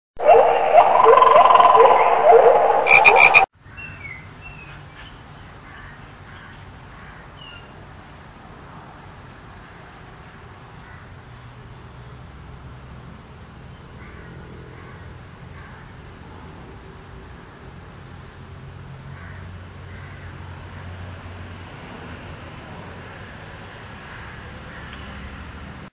Caterpillars eating a leaf
Caterpillars eating a leaf. In this short nature video you'll be seeing two  caterpillars eating and munching on a leaf